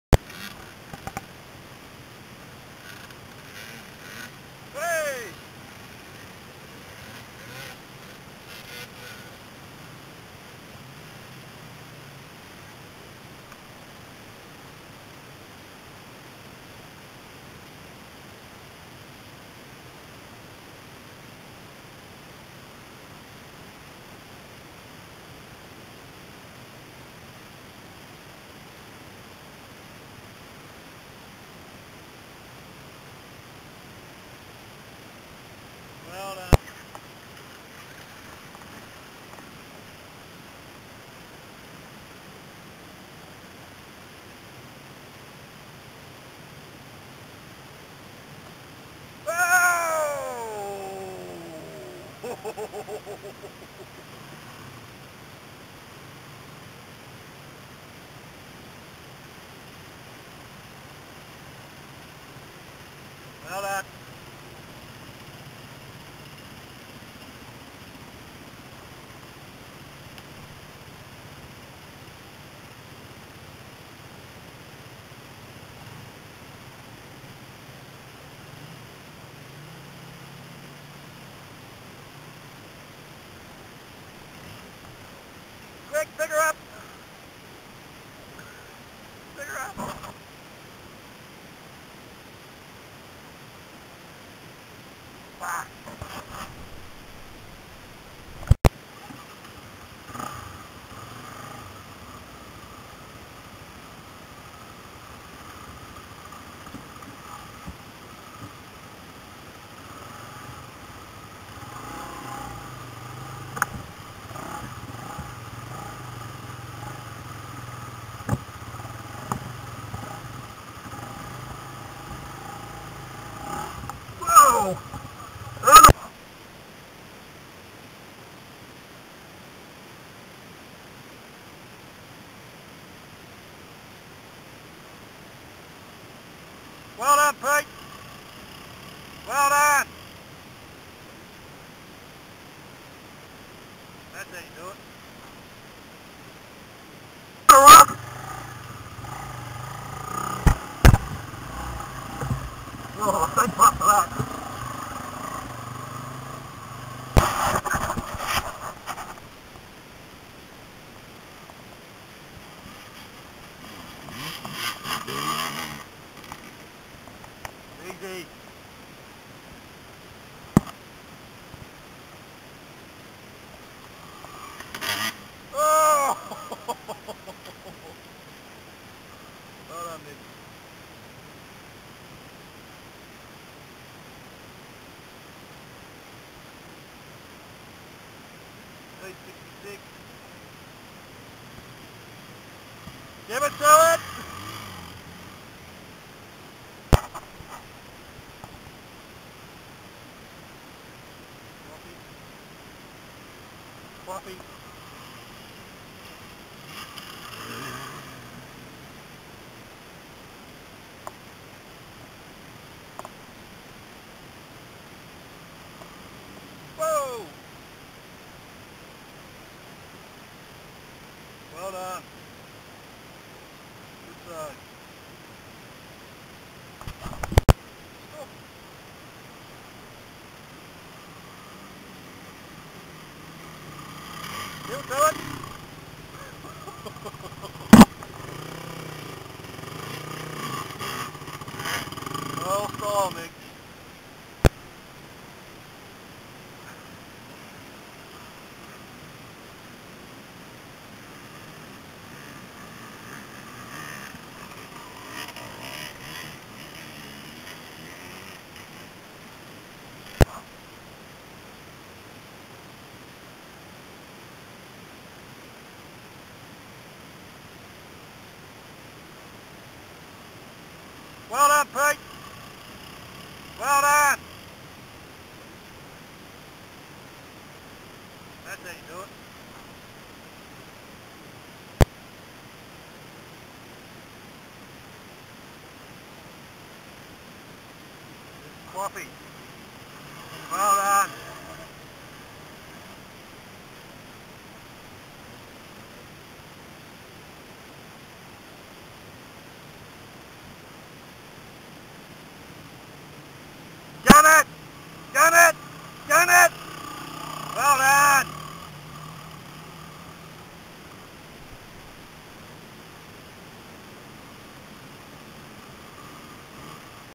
Neerim
Crossing the river at hawthorn and some of the tracks on the other side